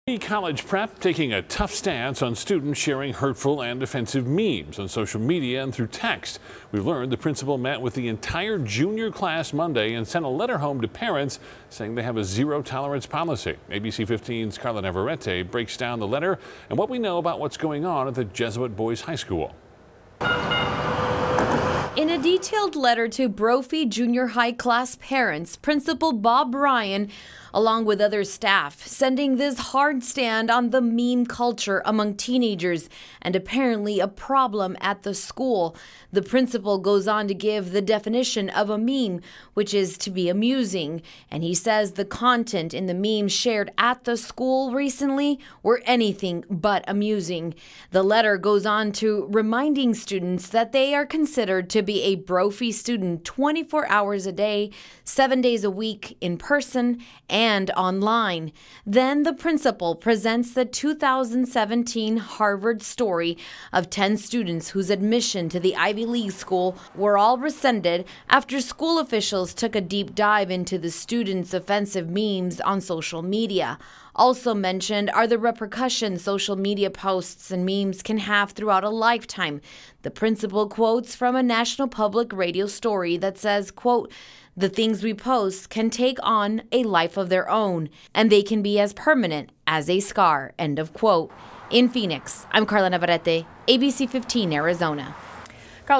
Brophy Prep takes a hard stand on offensive memes
School says they have a zero-tolerance policy with offensive social media posts.